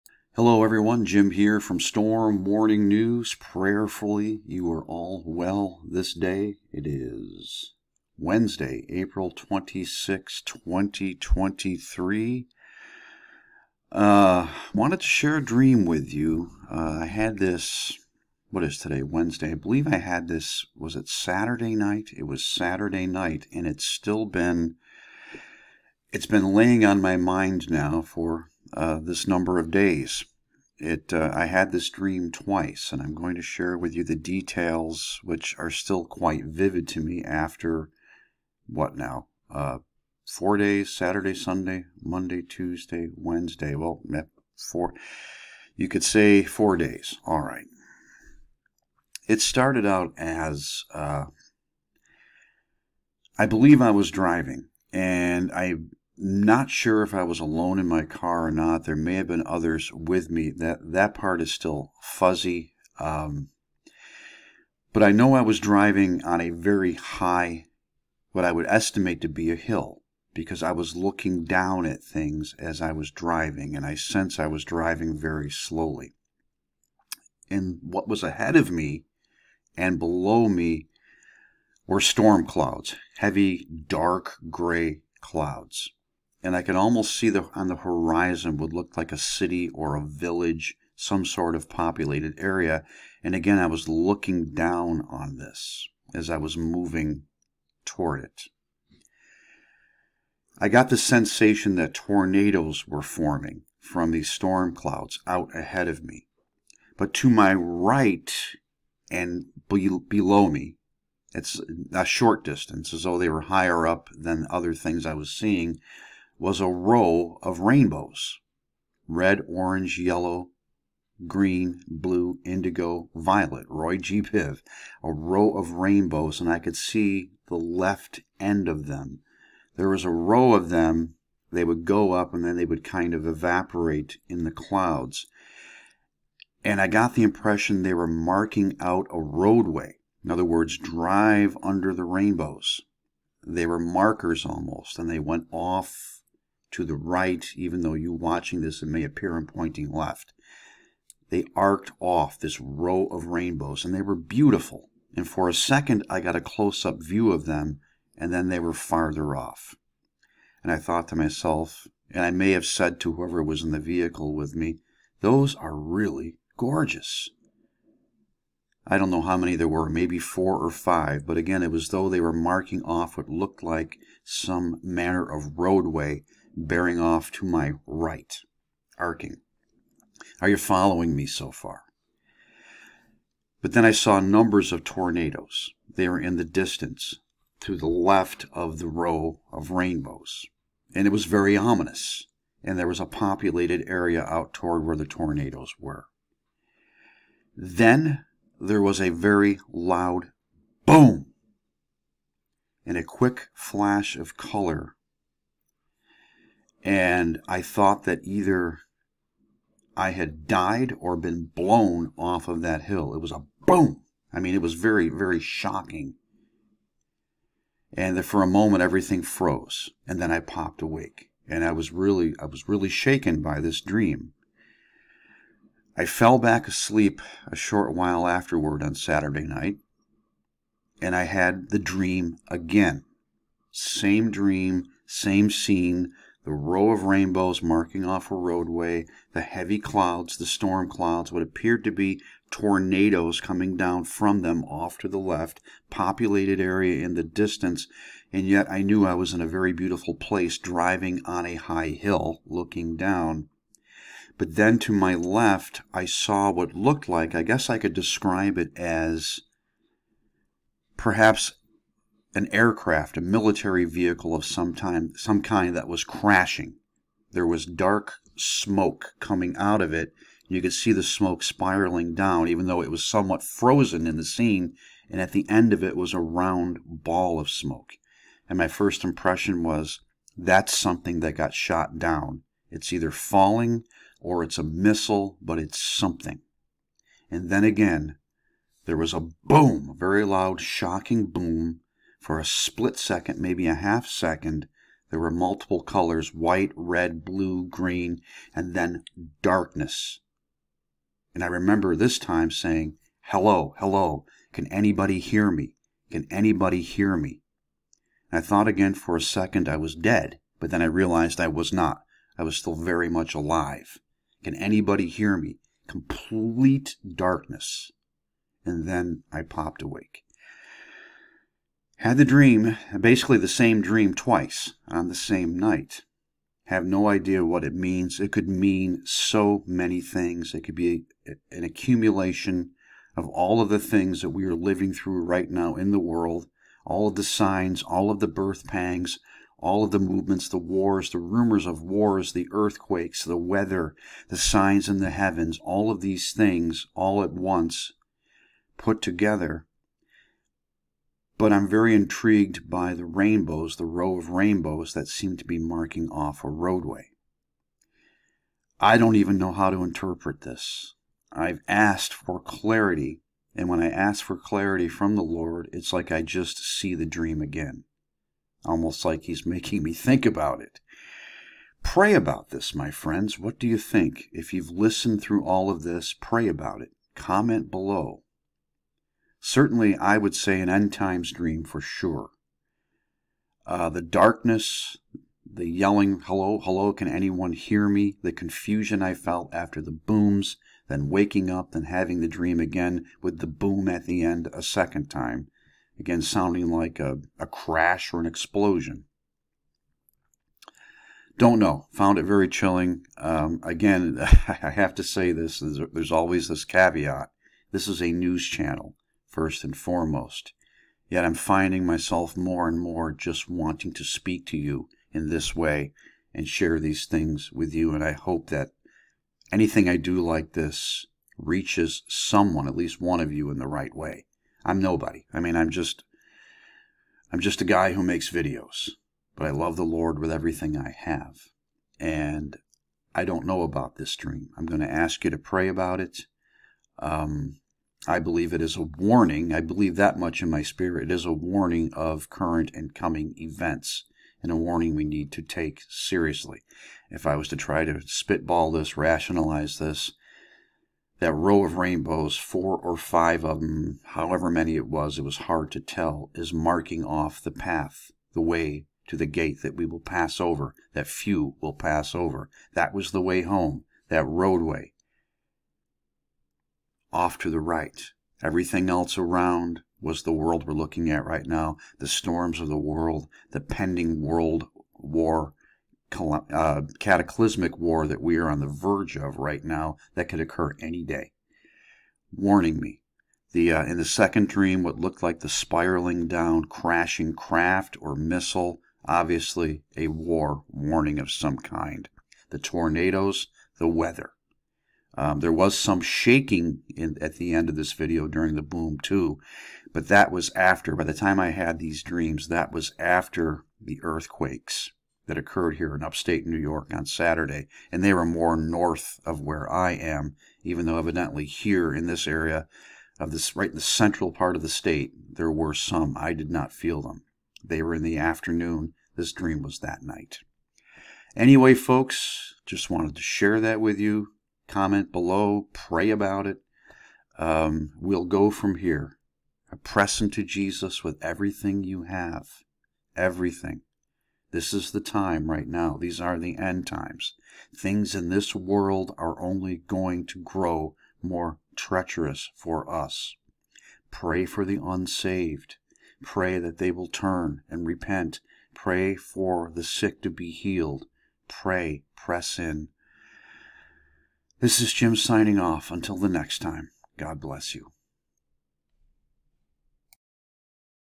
Strange Dream Twice: Row of Rainbows, High Hill, Storm Clouds, Crashing Jet or Missile
This has been on my mind since 04-22-23. Praying for clarity. God bless you all.

End-Times Fiction book series 'The Wayfarers Stories' available in Kindle and Print on Amazon